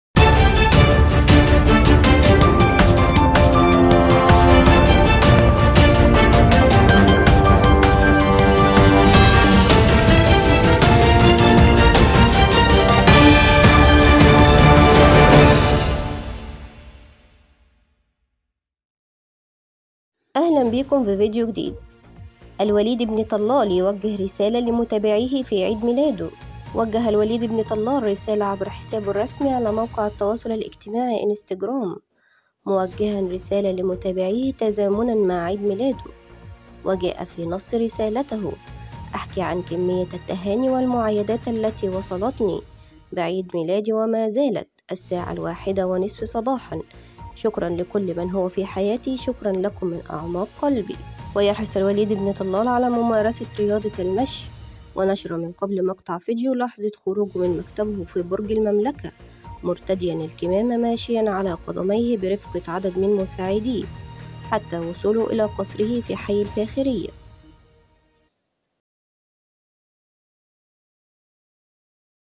رساله الامير الوليد بن طلال يوجه رساله لمتابعيه في عيد ميلاده
نشر الامير الوليد بن طلال  صورة عبر حسابه الرسمى على موقع التواصل الاجتماعى إنستجرام، موجها رسالة لمتابعيه تزامنا مع عيد ميلاده.

وجاء في نص رسالته: "احكي عن كمية التهاني والمعايدات اللي وصلتني بعيد ميلادي وما زالت الساعة الواحدة ونصف صباحا شكرا لكل من هو في حياتي شكرا لكم من اعماق قلبي".